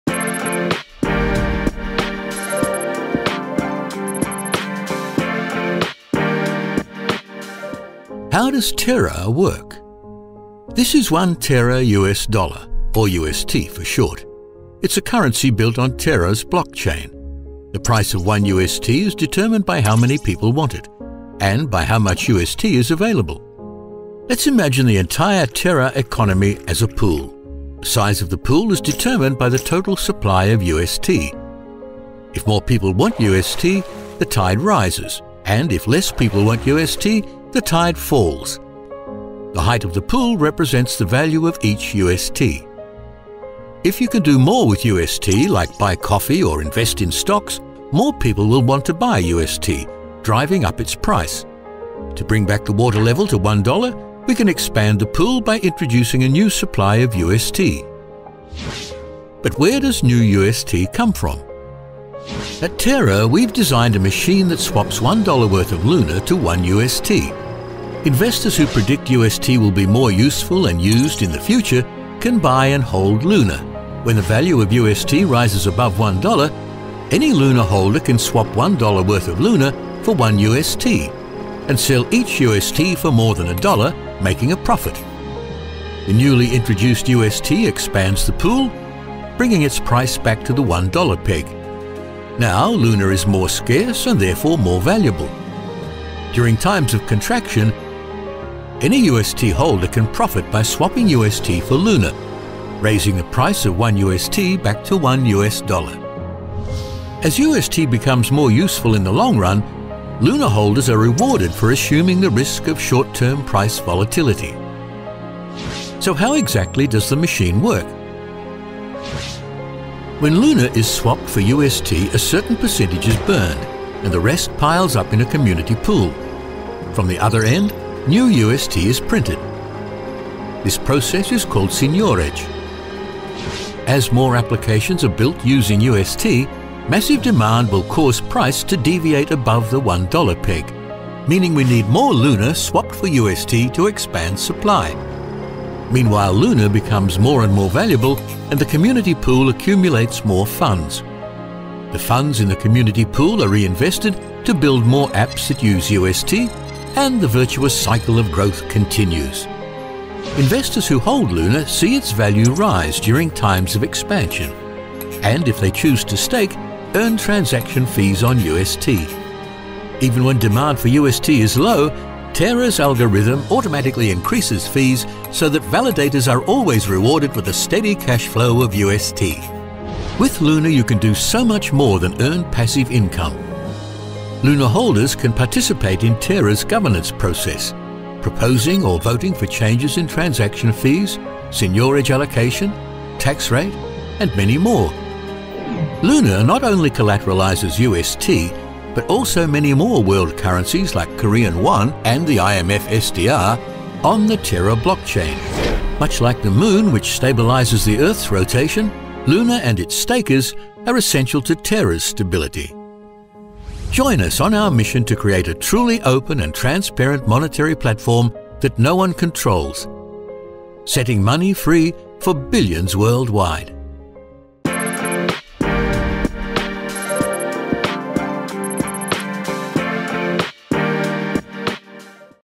What is Terra, Luna, Anchor?
This video explains the Terra project. We are grateful to the Terra team for supporting hack.harmony.one $1 Million hackathon. To learn more visit